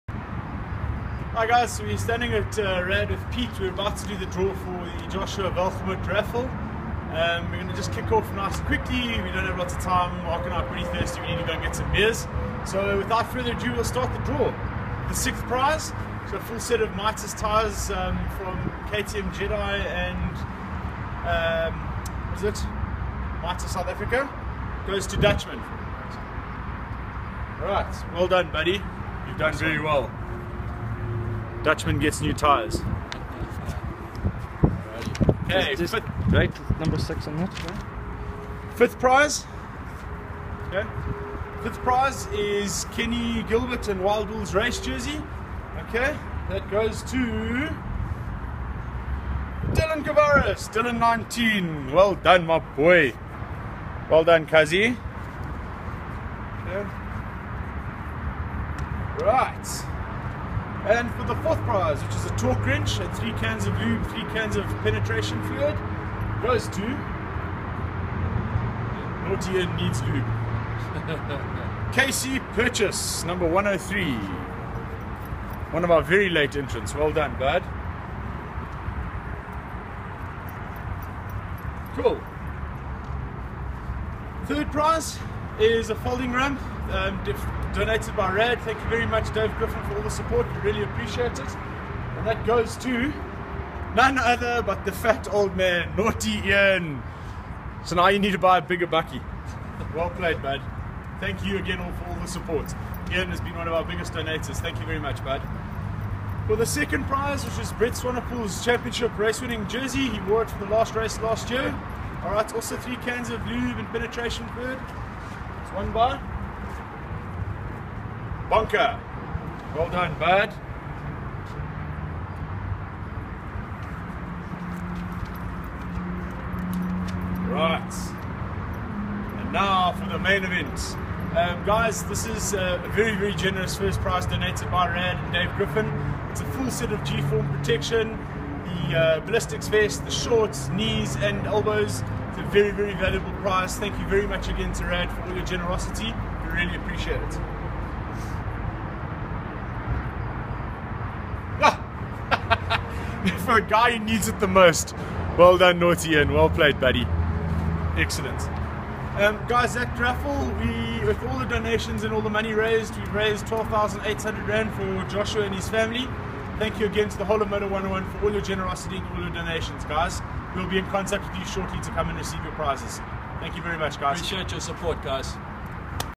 Moto101 Joshua Raffle Draw
Thanks to RAD Moto, Kenny Gilbert, Wild Will, Brett Swanepoel, Lazer Airbrakes, and Mitas SA for helping us out with donations in our fund raising raffle. We managed to raise R 12800.00 for young Joshua and his family to help their battle with cancer.

Congrats to the raffle winners as well!

1. The G-Form Protection Set + 3 cans of chain lube and penetrating spray - Naughty Ian
2. Brett Swanepoels Jersey + 3 cans of chain lube and penetrating spray - Bonker
3. Alu Folding Bike Ramp + 3 cans of chain lube and penetrating spray - Naughty Ian
4. Torque Wrench + 3 cans of chain lube and penetrating spray - KC Purchase
5. Kenny and Will's jerseys - Dillan19
6. Set of Mitas Tyres - Dutchman

Thanks to you all on Moto101 for helping make a difference.